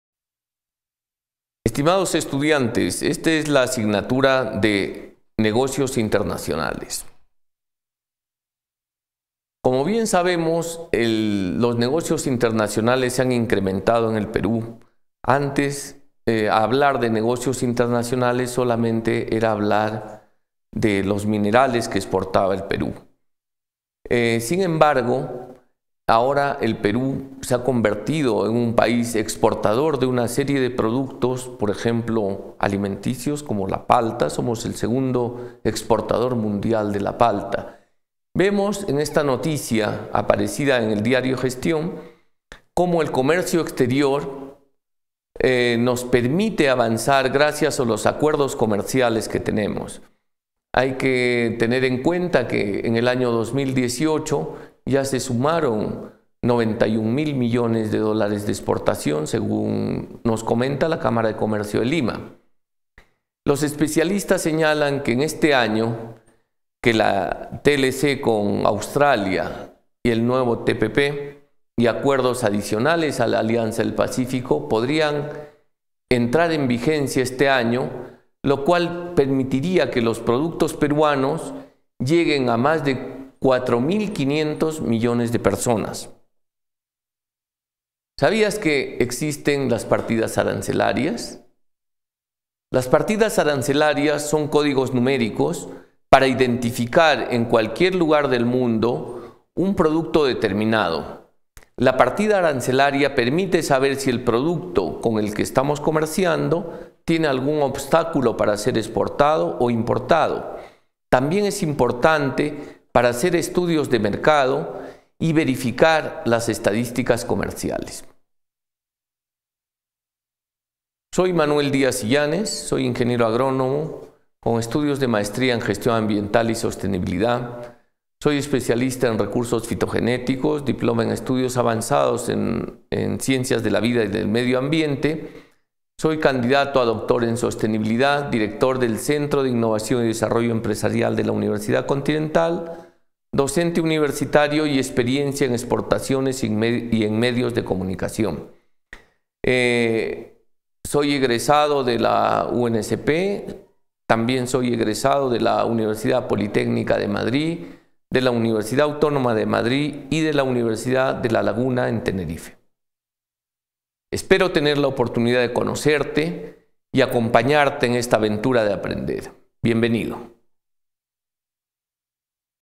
Presentación de la asignatura: Negocios Internacionales
Manuel Díaz Illanes docente de la asignatura.